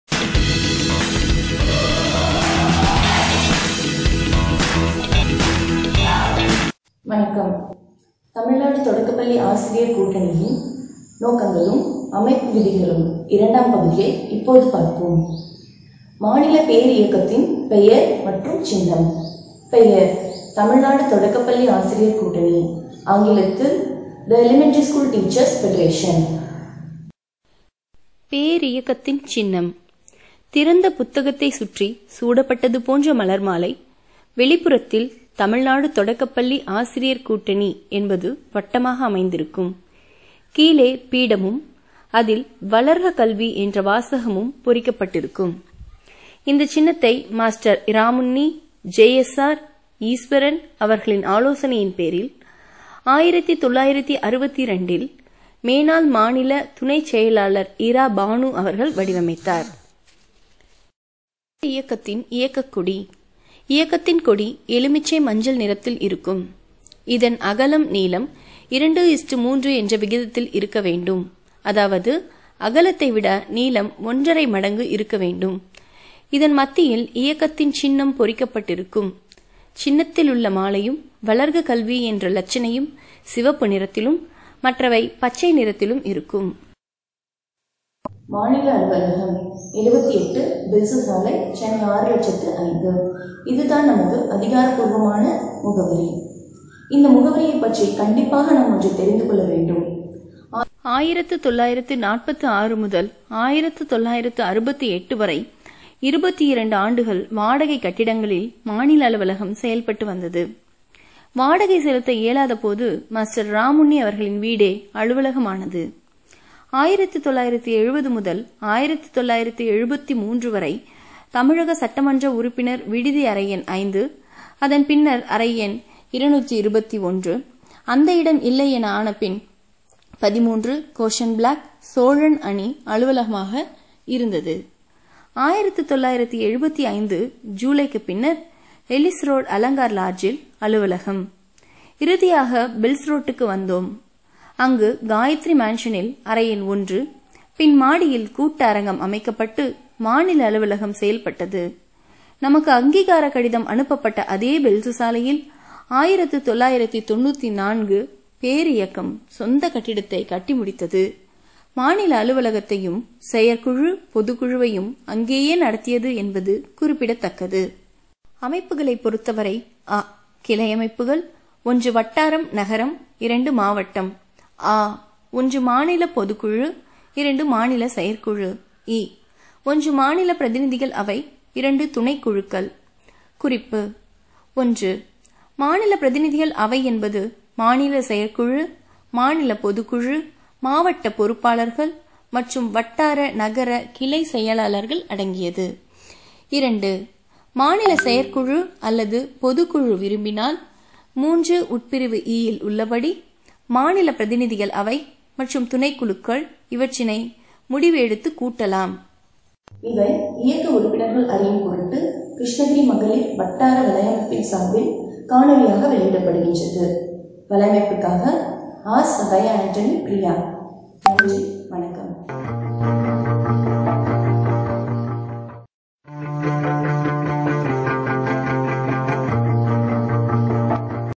பகுதி 5 - பேரியக்கத்தின் -மாநில அமைப்பு பெயர் மற்றும் சின்னம் - நோக்கங்களும் அமைப்பு விதிகளும்
வீடியோ –
வணக்கம்
தமிழ்நாடு தொடக்கப்பள்ளி ஆசிரியர் கூட்டணியின்
நோக்கங்களும் அமைப்பு விதிகளும்
இரண்டாம் பகுதியை
இப்போது பார்ப்போம்
மாநில அமைப்பு
பேரியக்கத்தின் . பெயர் மற்றும் சின்னம்
பெயர் -தமிழ்நாடு தொடக்கப்பள்ளி ஆசிரியர் கூட்டணி

ஆங்கிலத்தில்

THE TAMILNADU ELEMENTARY SCHOOL TEACHERS FEDERATION

ஆடியோ - பேரியக்கத்தின்  சின்னம்

திறந்த புத்தகத்தைச் சுற்றி சூடப்பட்டது போன்ற மலர் மாலை, வெளிப்புறத்தில் “தமிழ்நாடு தொடக்கப்பள்ளி ஆசிரியர் கூட்டணி” என்பது
வட்டமாக அமைந்திருக்கும். கீழே பீடமும் அதில் “வளர்க கல்வி'” என்ற வாசகமும் பொறிக்கப்பட்டு இருக்கும்.

இந்த சின்னத்தை மாஸ்டர் இராமுண்ணி , ஜே.எஸ்.ஆர் ,ஈஸ்வரன் அவர்களின் ஆலோசனையின் பேரில் 1962ல் மேனாள் மாநில துணைச்செயலர் இரா. பானு அவர்கள் வடிவமைத்தார் .

பேரியக்கத்தின் இயக்கக் கொடி

இயக்கத்தின் கொடி (எலுமிச்சை) மஞ்சள் நிறத்தில் இருக்கும். இதன்
அகலம் நீளம் 2 : 3 என்ற விகிதத்தில்  இருக்க வேண்டும் அதாவது அகலத்தை விட நீளம் ஒன்றரை மடங்கு இருக்கவேண்டும் . இதன் மத்தியில் இயக்கத்தின் சின்னம் பொறிக்கப்பட்டிருக்கும். சின்னத்திலுள்ள மாலையும் “வளர்க கல்வி என்ற இலச்சினையும் சிவப்பு நிறத்திலும் மற்றவை பச்சை நிறத்திலும் இருக்கும்.

2. மாநில அலுவலகம்
78, பெல்சு சாலை, சென்னை - 600 005.
இந்த முகவரிதான் நமது அதிகாரபூர்வமான முகவரி

இந்த முகவரிப்பற்றி கண்டிப்பாக நாம் ஒன்றினை தெரிந்துகொள்ள வேண்டும்

1946 முதல் 1968 வரை 22 ஆண்டுகள் வடகை கட்டிடங்களில் மாநில அலுவலகம் செயல்பட்டு வந்தது . வாடகை செலுத்த இயலாதபோது   மாஸ்டர் இராமுண்ணி அவர்களின் வீடே அலுவலகமானது..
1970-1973 வரை  தமிழக சட்டமன்ற உறுப்பினர் விடுதி அறை எண் 5 அதன் பின்னர் அறை எண் 221  அந்த இடம் இல்லை என ஆன பின் 13 கோஷன் பிளாக் சோழன் அணி அலுவலகமாக இருந்தது. 1975 ஜீலைக்கு பின்னர் எலிஸ்ரோடு அலங்கார் லாட்ஜில் அலுவலகம் இறுதியாக  பெல்ஸ் ரோடுக்கு வந்தோம் அங்கு காயத்திரி மான்சனில் அறை எண் 1. பின் மாடியில் கூட்டஅரங்கம் அமைக்கப்பட்டு மாநில அலுவலகம் செயல் பட்டது .நமக்கு அங்கீகாரகடிதம் அனுப்பப்ட்ட அதே பெல்சு சாலையில் 1994 பேரியக்கம் சொந்த கட்டிடத்தை கட்டி முடித்து மாநில அலுவலகத்தையும் செயற்குழு பொதுக்குழுவையும் அங்கேயே நடத்தியது.என்பது குறிப்பிட தக்கது
 அமைப்புகளை பொருத்தவரை
(அ) கிளை அமைப்புகள்
1. வட்டாரம் / நகரம்
2. மாவட்டம் (வருவாய்)

(ஆ) 1. மாநிலப் பொதுக்குழு
          2. மாநில செயற்குழு

(இ) 1. மாநில பிரதிநிதிகள் அவை
        2.        துணைக்குழுக்கள்

குறிப்பு : 1. மாநிலப் பிரதிநிதிகள் அவை என்பது மாநிலச் செயற்குழு,
மாநிலப் பொதுக்குழு, மாவட்டப் பொறுப்பாளர்கள் மற்றும் வட்டார / நகரக்
கிளைச் செயலாளர்கள் அடங்கியது.

2. மாநில செயற்குழு அல்லது பொதுக்குழு விரும்பினால் 3 உட்பிரிவு இ -- ல் உள்ளபடி மாநில பிரதிநிதிகள் அவை   மற்றும்         துணைக்குழுக்கள்
இவற்றினை முடிவு எடுத்துக் கூட்டலாம் .

வீடியோ - இவை இயக்க உறுப்பினர்கள் அறியும் பொருட்டு கிருஷ்ணகிரி வட்டார மகளிர் வலையமைப்பு . சார்பில் காணொலியாக வெளியிடப்படுகிறது மகளிர் வலையமைப்புக்காக ………சகாயஅண்டணி பிரியா …. . நன்றி வணக்கம்